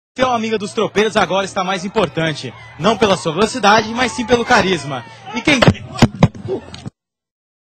Mule Reporter
Who knew mule racing could be so interesting? Tell this tool he should do his reporting from the stands...not from the middle of the track.